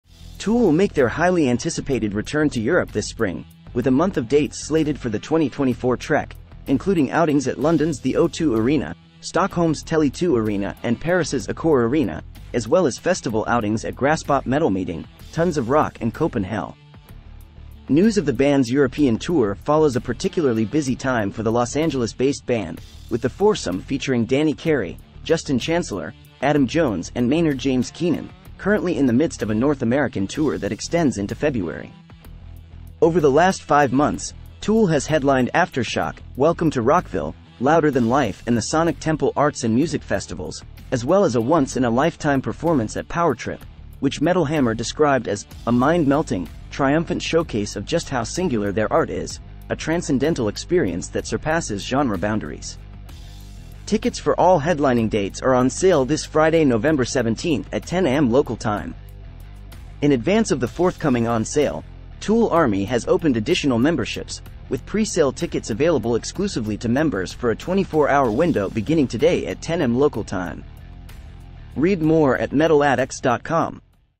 TOOL Reveals Dates For 2024 European Tour
TOOL European tour dates:

25 May Hannover, DE ZAG Arena

27 May  Amsterdam, NL Ziggo Dome

30 May Birmingham, UK Resorts World Arena

1 June Manchester, UK AO Arena

3 June London, UK The O2 Arena

5 June Paris, FR Accor Arena

8 June Berlin, DE Parkbühne Wuhlheide

10 June Wien, AT Wiener Stadthalle

11 June Kraków, PL Tauron Arena

13 June Budapest, HU BudapestAréna

18 June Köln, DE Lanxess Arena

20 June Dessel, BE Graspop Metal Meeting

22 June Copenhagen, DK CopenHell

25 June Stockholm, SE Tele2 Arena

27 June Oslo, NO Tons of Rock